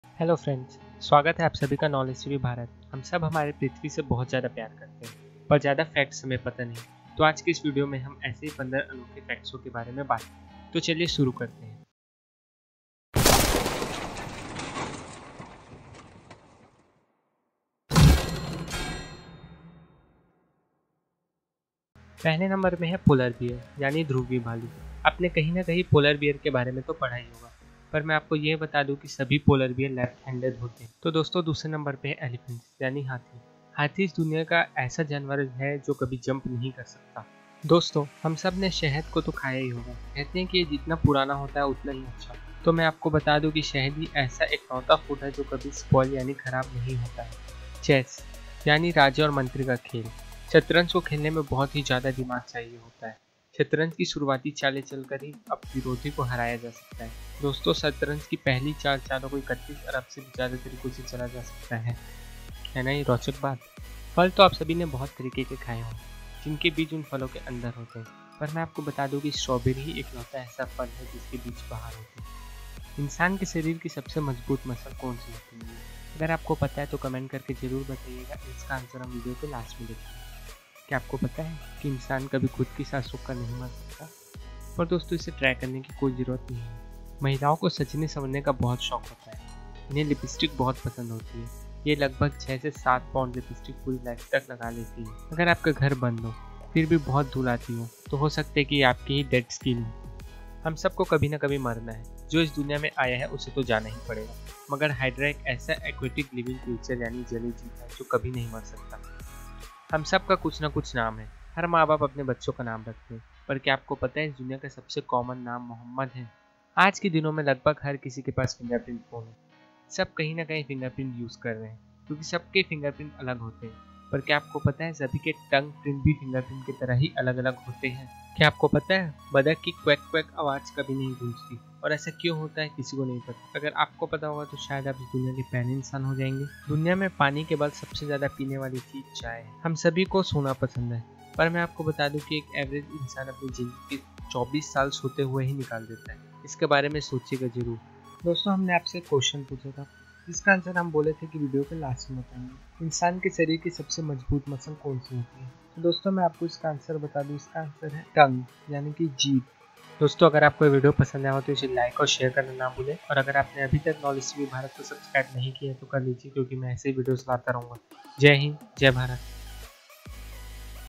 दुनिया के 15 सच जो आप नहीं जानते (Part 1) ||15 Intresting Facts|| You dont know|| 2019
Hi

Dosto Hum is video mai duniya ke 15 intresting amazing aur rochak fasts ke baare mai baat krenge jisse aap shayad hi jante honge is video mai hum desh duniya khelkhud se leke lagbhag sabhi cheezo ke baare mai dkhenge

पहली बार जानें इतने खास रोचक तथ्य.

Dosto is channel pe aapko aviation, history, religious, intresting, se related videos hindi me dekhne ko milte rhegi.

Knowledge Tv Bharat
Knowledgetvbharat